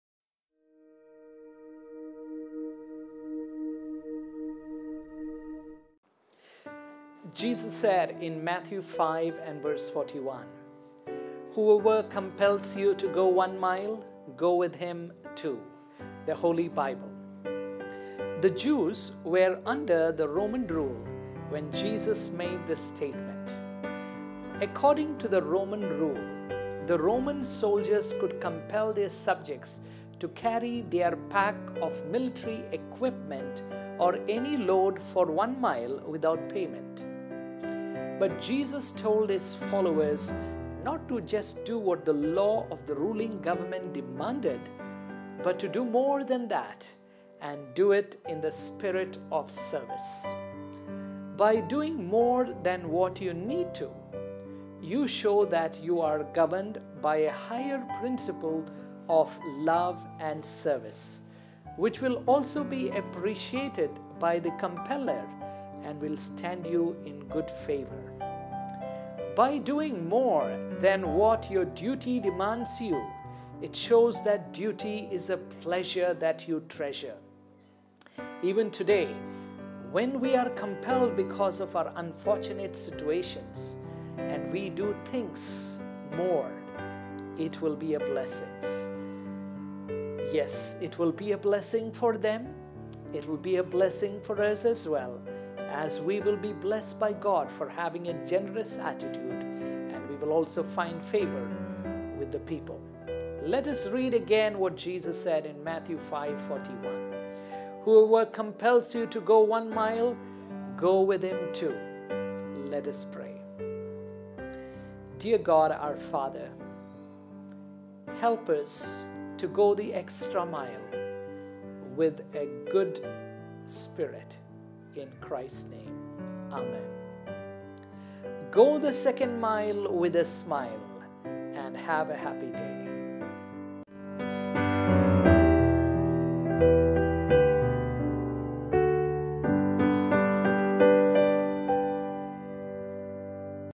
Going the Second Mile with a Smile! (2 mins.) Reflection for Today, by Pr. Michael Pedrin
Why to Go the 2nd Mile?